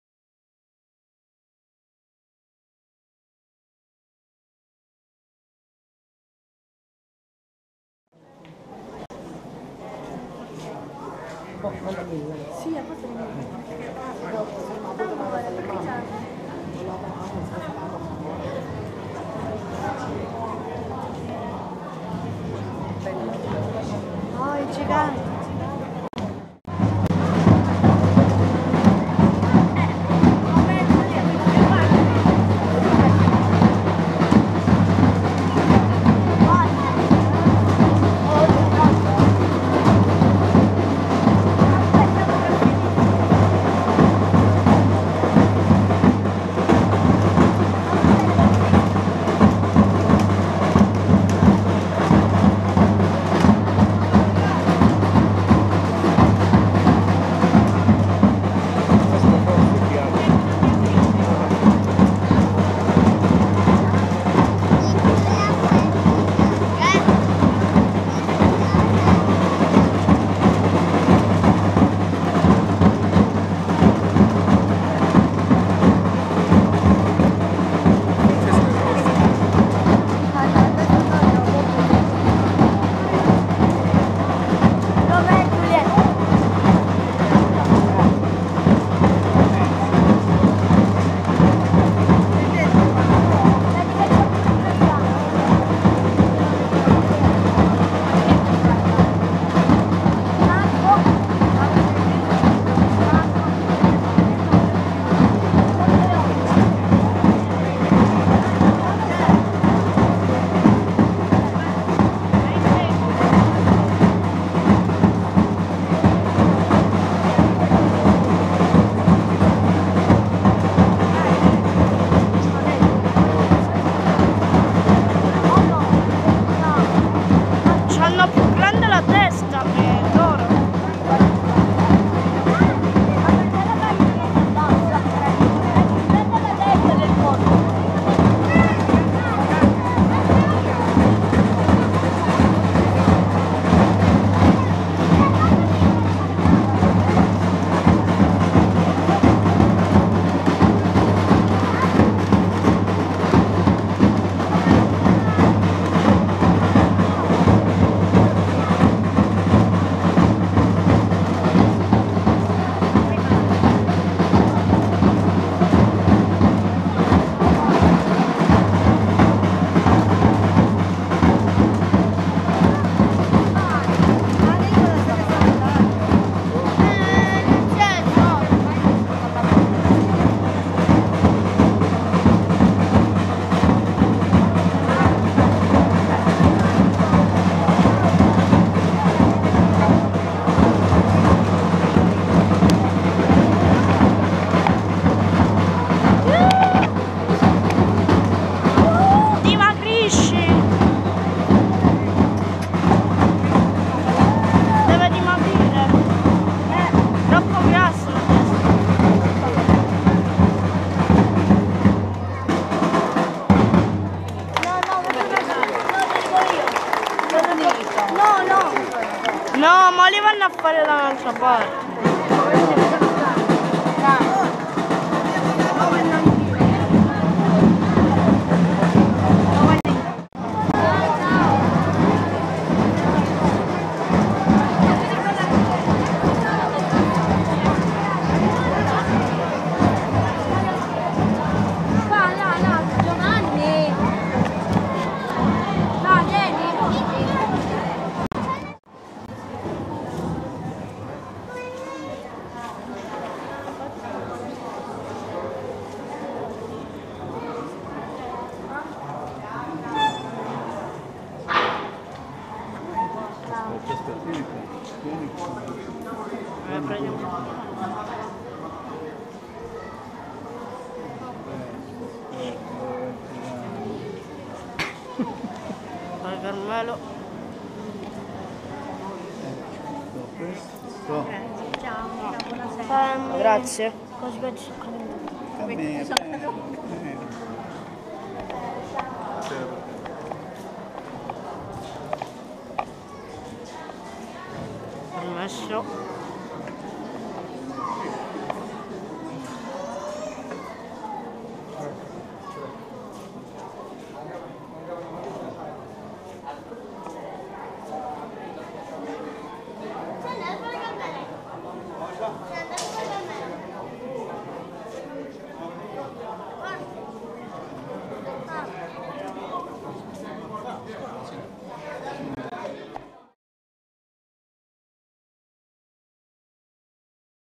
parte 7 GoPro - Processione Assunta
Processione di Maria SS. Assunta in cielo del 15 agosto 2015 a Stefanaconi.